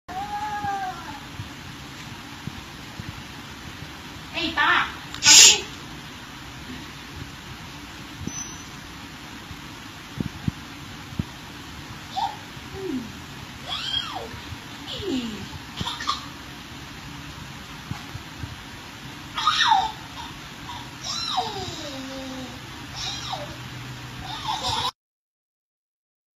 Video dera viral, pembantu rumah didakwa
Seorang pembantu rumah di Singapura didakwa di mahkamah hari ini selepas rakaman video penderaaan anak majikannya viral di media social Facebook.

Beliau didakwa di bawah Akta Kanak-kanak dan rang Muda.